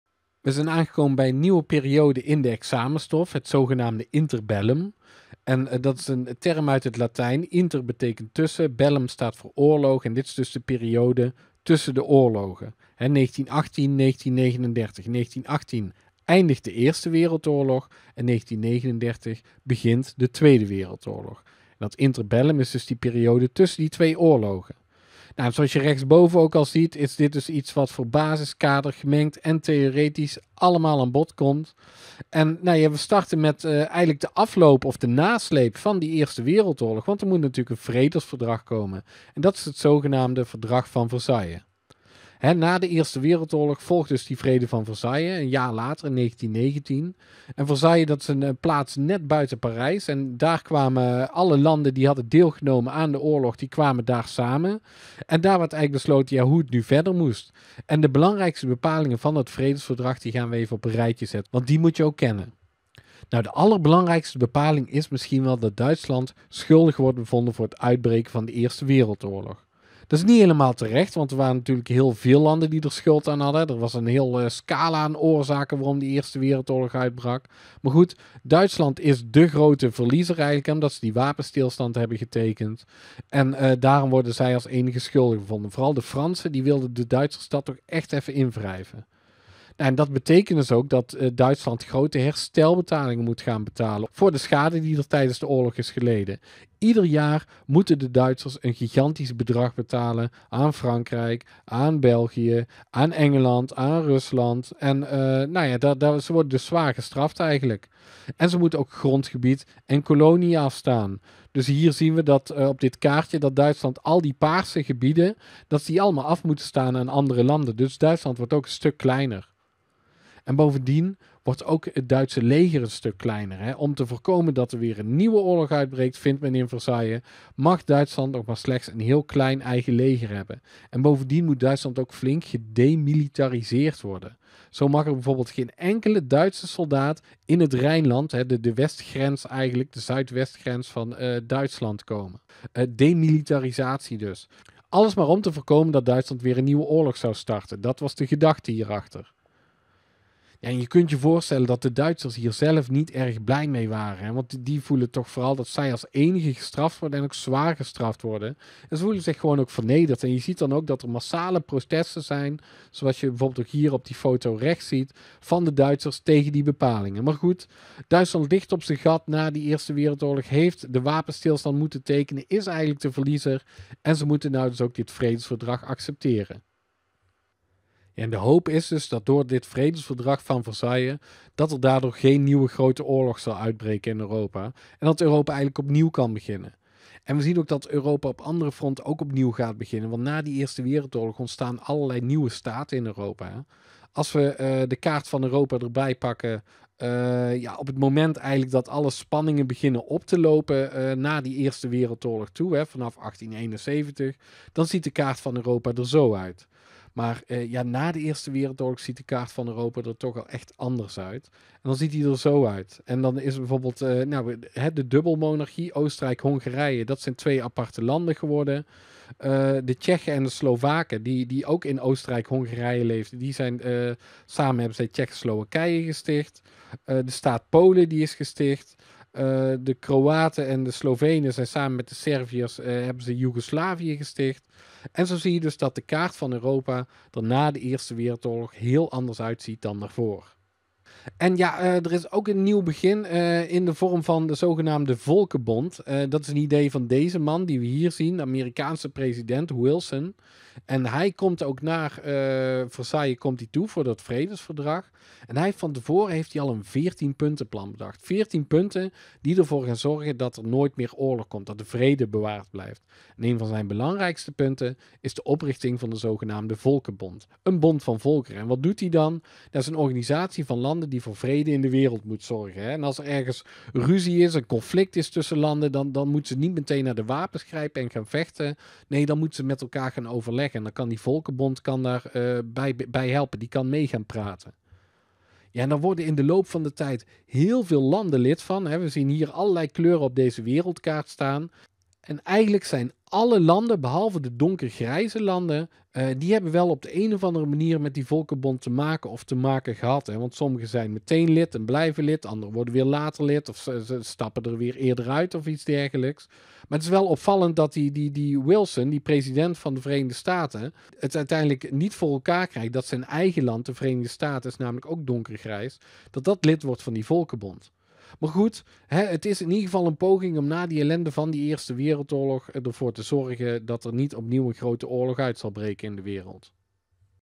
1. De vrede van Versailles (vmbo eindexamen - Het interbellum 1918-1939)
Deze video hoort bij het onderwerp 'het interbellum' van het eindexamen geschiedenis voor vmbo.
In deze video wordt uitgelegd:
- wat er besloten werd in de vrede van Versailles,
- wat de Volkenbond was.

Je vindt alle video's voor het vmbo-eindexamen op mijn kanaalpagina bij 'VMBO - Historisch overzicht'.

Afbeeldingen:
- eigen collectie